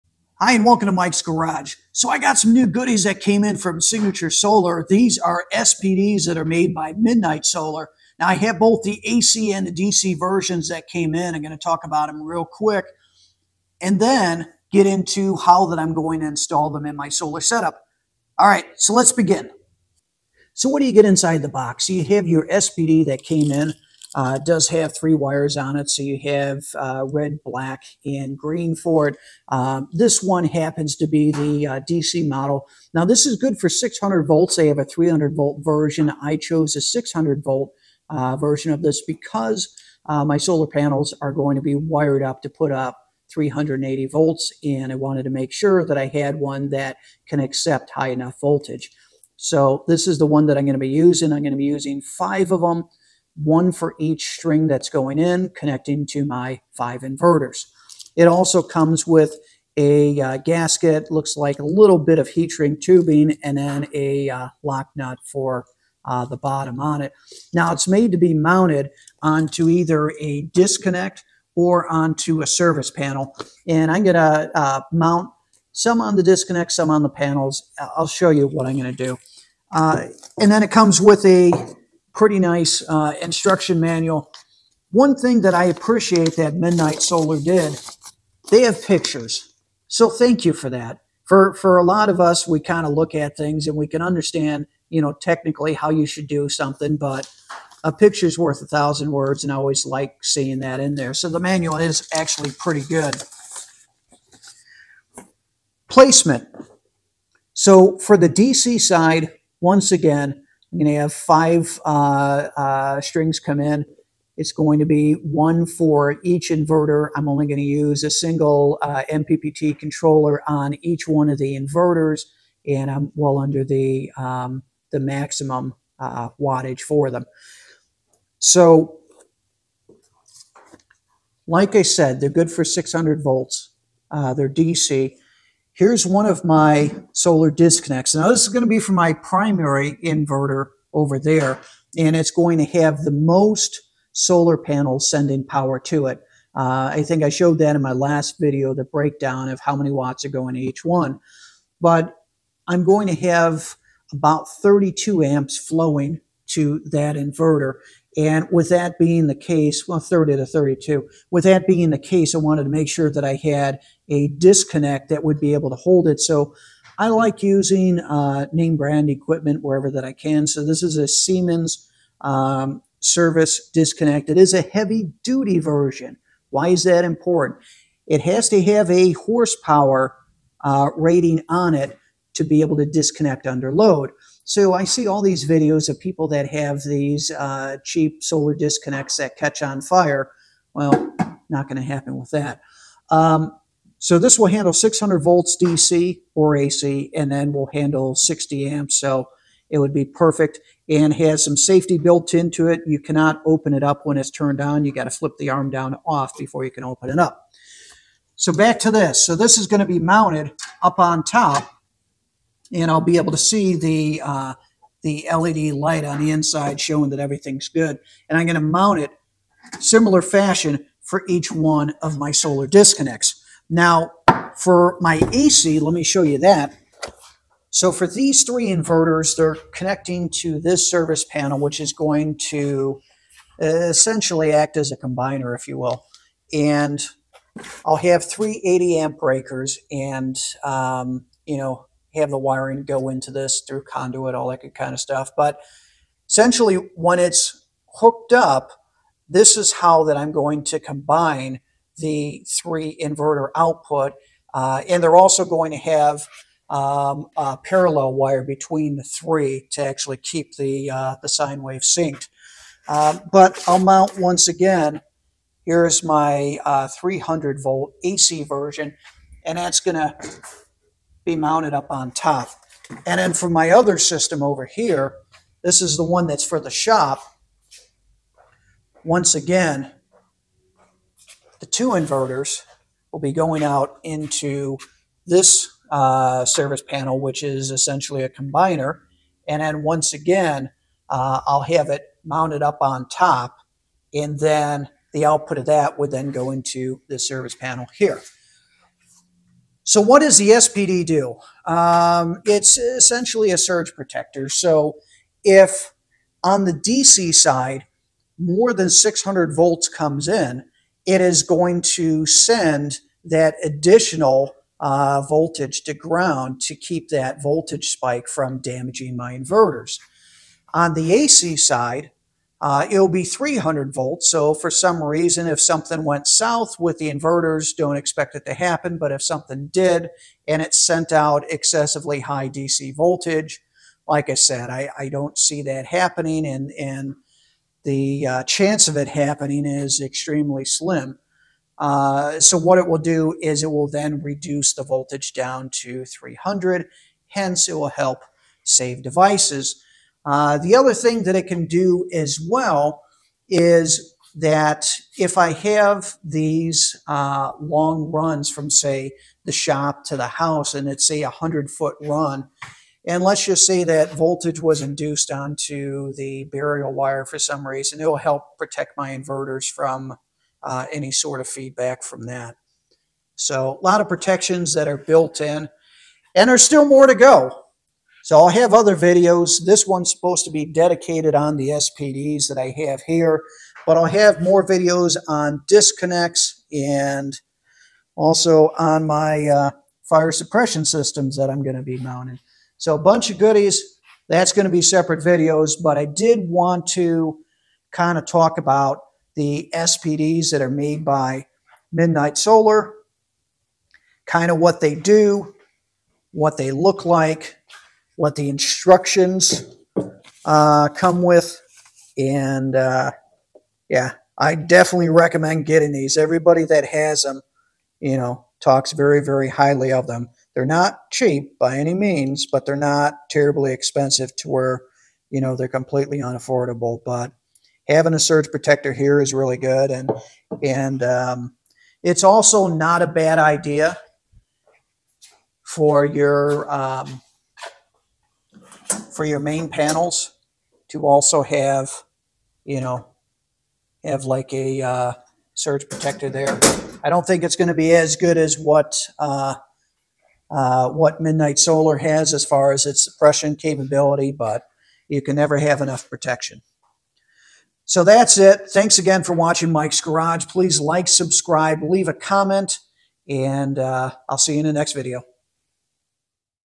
Surge Protection: The Last Line of Defense for Off-Grid Solar
Protecting your off-grid or hybrid solar investment is crucial, and that's why I'm sharing why I chose MidNite Solar SPDs (Surge Protective Devices) for my setup, all purchased from Signature Solar! In this video,

I'll explain why I bought both the MidNite Solar 600V DC SPDs and the MidNite Solar 300V AC SPDs.

These vital devices offer protection for both the DC solar input to your inverter and the AC output between your inverter and service panel. Adding this extra layer of defense is key to lengthening the useful life of your valuable inverter and safeguarding it against damaging lightning strikes or other high-voltage spikes. Don't skip this critical component for your solar system!